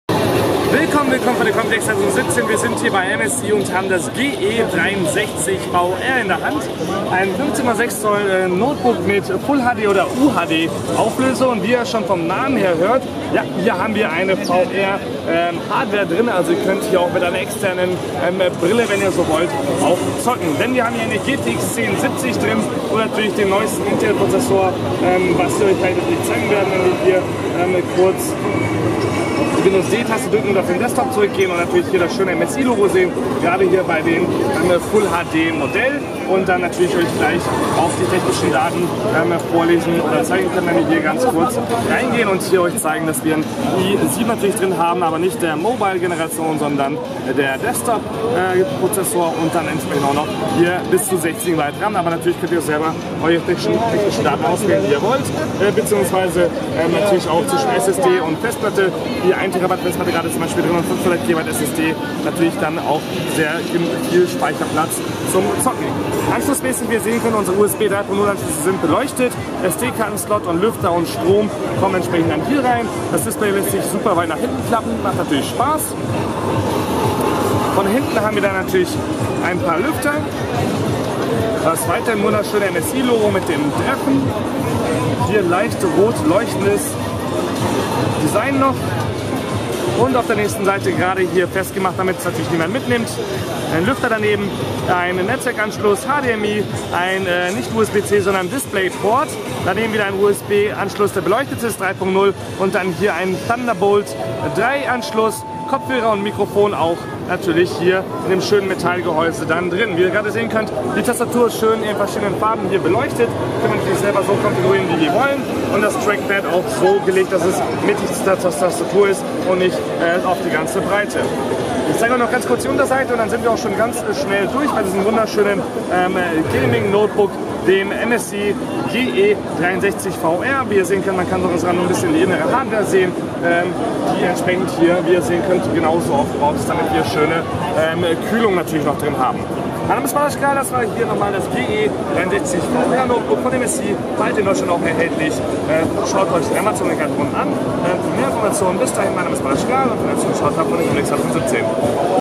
VR Notebook mit 120Hz Display | MSI GE63VR | Erster Eindruck
Kaufen - MSI hat auf der Computex 2017 Messe in Taiwan einige Neuigkeiten vorgestellt. Dazu gehört der MSI GE63VR mit nicht nur atemberaubender Leistung, sodass wir auch externe eine VR Brille sogar verwenden können, sondern sogar ein neues schnelles Display für Gamer ist verbaut. Mit 120Hz erkennen wir noch schneller und präziser die Abläufe in Spielen und vielen anderen Umgebungen auch.

Dieses Video wurde in 4K UHD aufgezeichnet und beinhaltet keine bezahlte Produktwerbung.

Meinen Flaschenöffner Ring gibt es hier für 2 Euro mit 2 Stück in der Packung zu kaufen